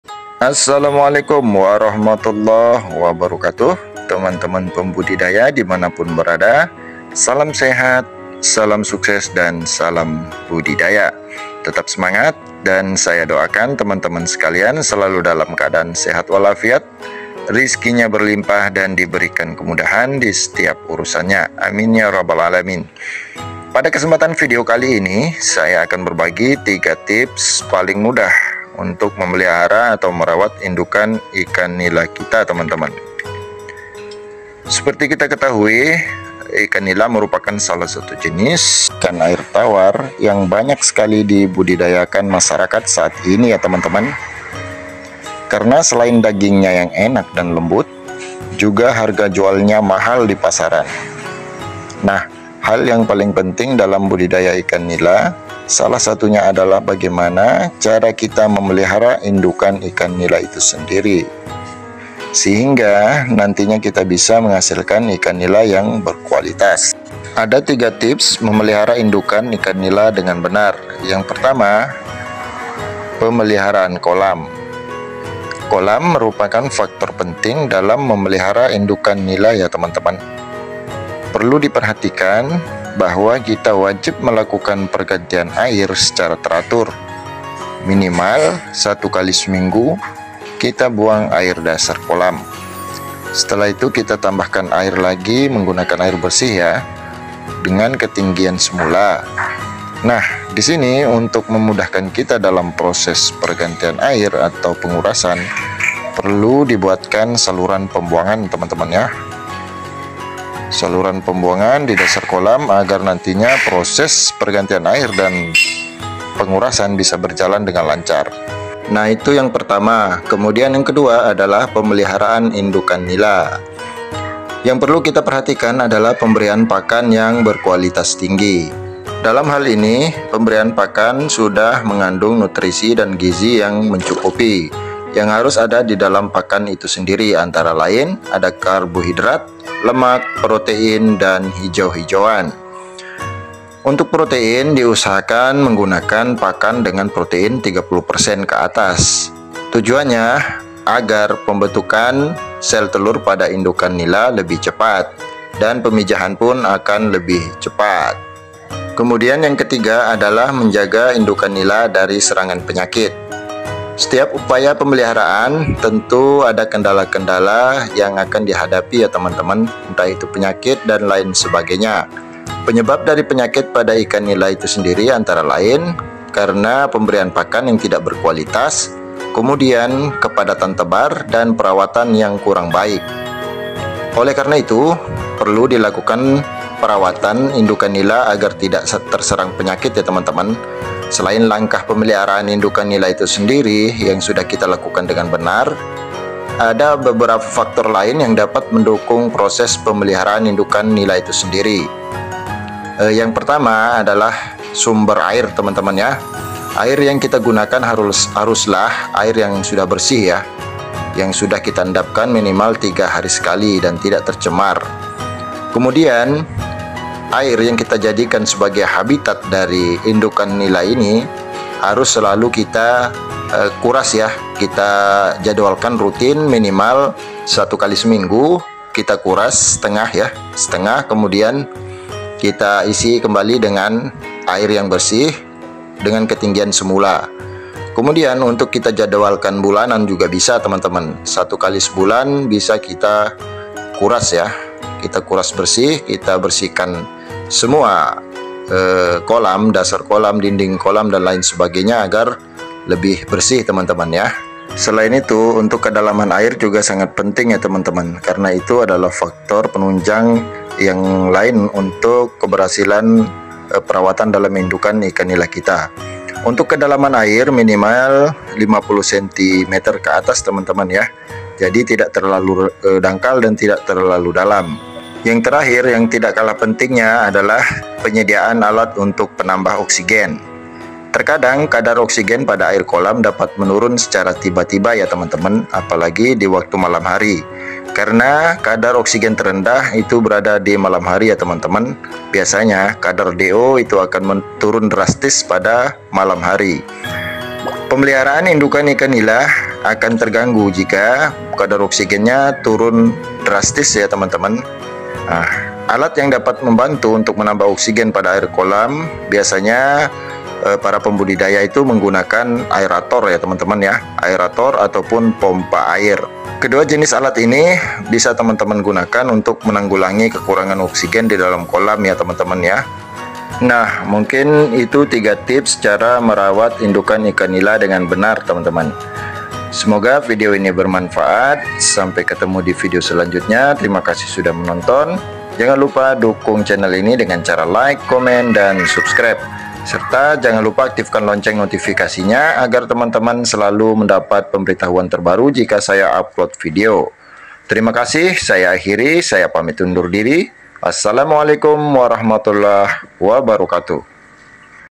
3Tips Mudah Memelihara Induk Ikan Nila
3Tips Mudah Merawat/memelihara Induk Ikan Nila
DI vidido ini saya menjelaskan 3 tips mudah merawat/pemeliharaan induk ikan nila.